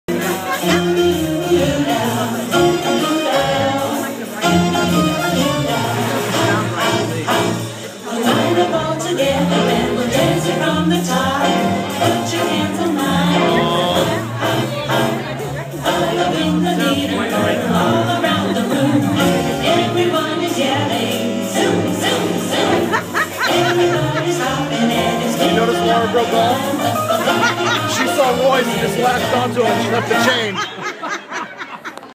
Bunny Hop
Danette, Tami, Cindy, Brittany and a little bit of Laura doing the bunny hop.